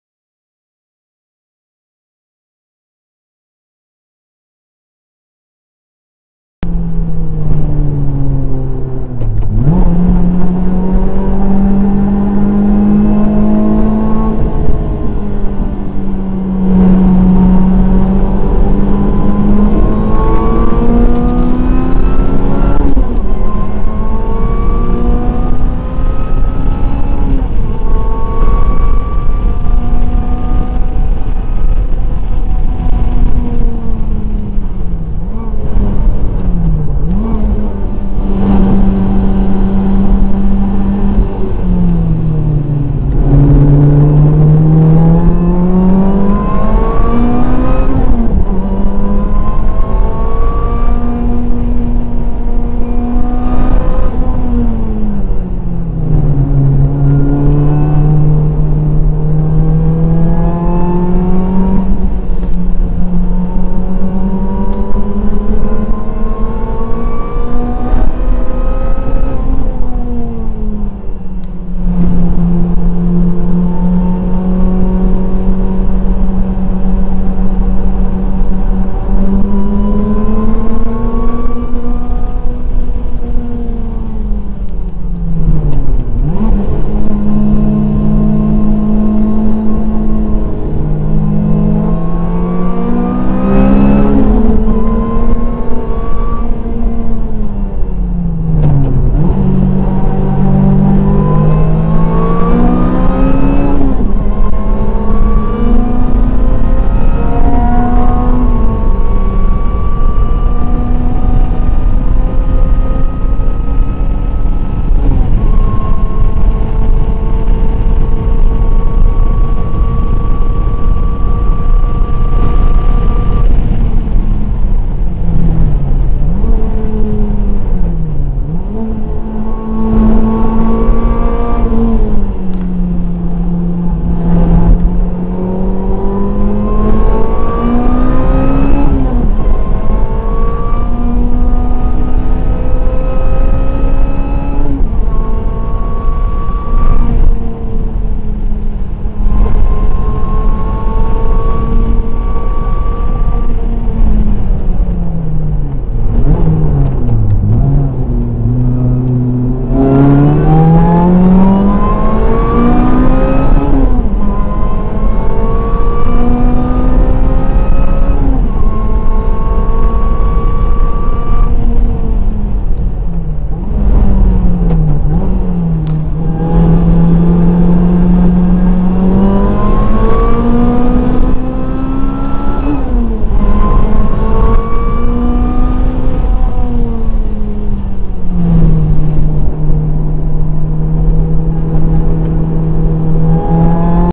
Lotus Challenge Series East Coast Exhibition @ Monticello
Lotus Challenge Series East Coast Exhibition Event at Monticello Motor Club in September 2010 - CodeBlue/Tax-Whistleblower.com Racing Supercharged Lotus Elise.  I absolutely love racing in the rain... and Hoosier Wets don't hurt either!